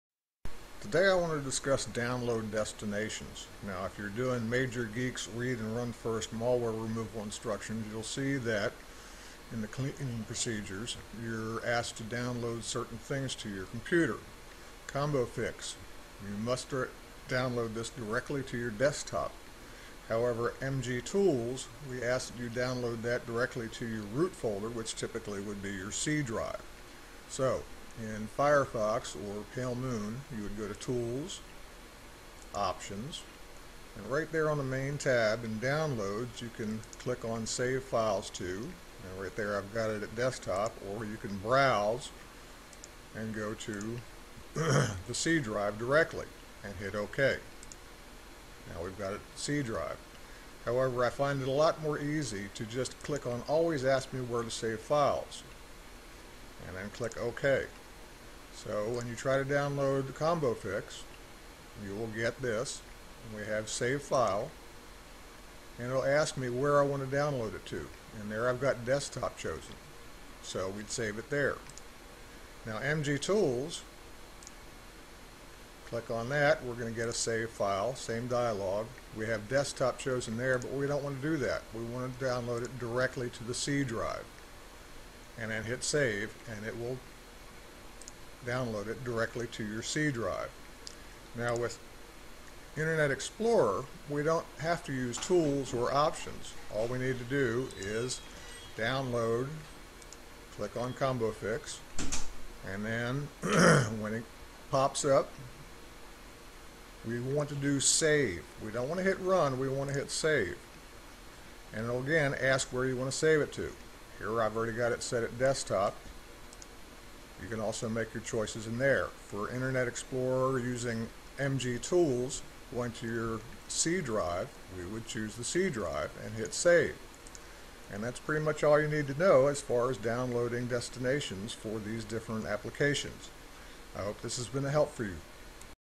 Downloading Malware Removal Tools by Majorgeeks.com
In this video, TimW shows you how to download the tools you need to follow our Malware Removal Guide